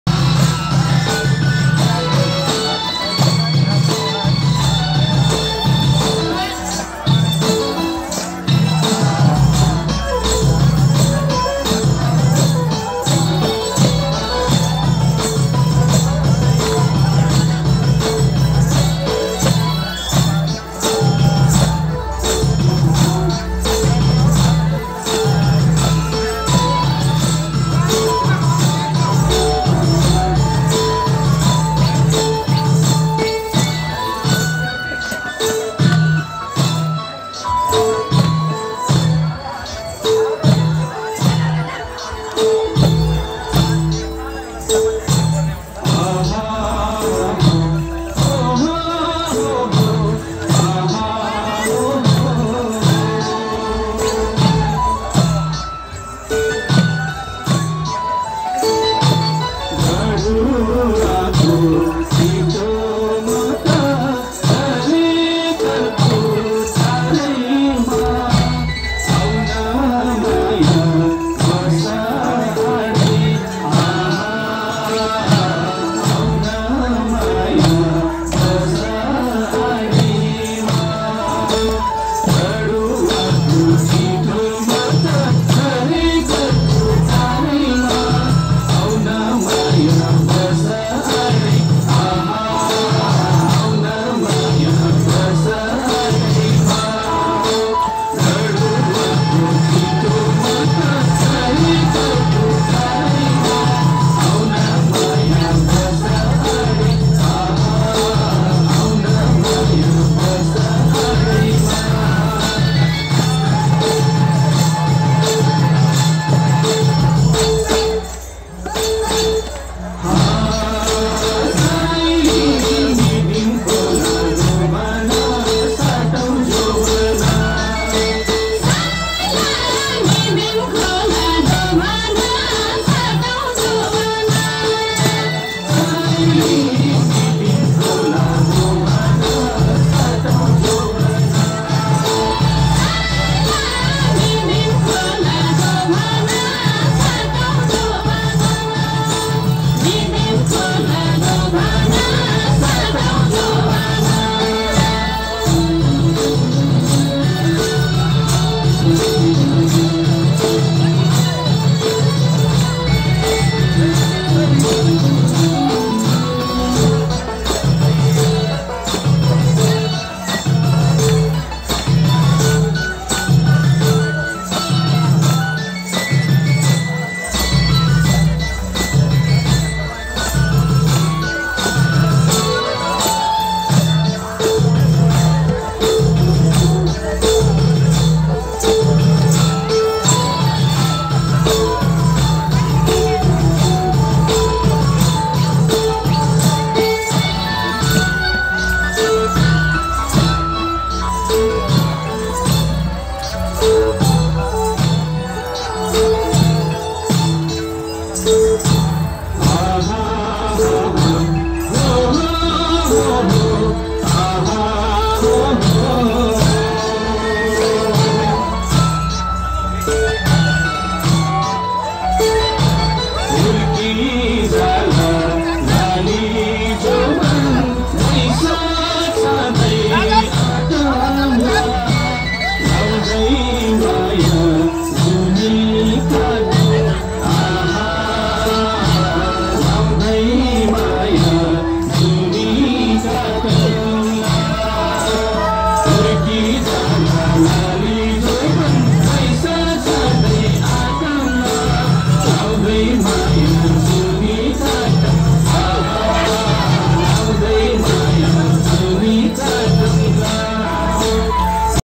Gurung Dance -2023 गुरुङ नृत्य | Pokhara
Ghatu dance is a Nepalese folk dance of the Gurung community of western Nepal. The dance is performed mainly during Baisakh Purnima.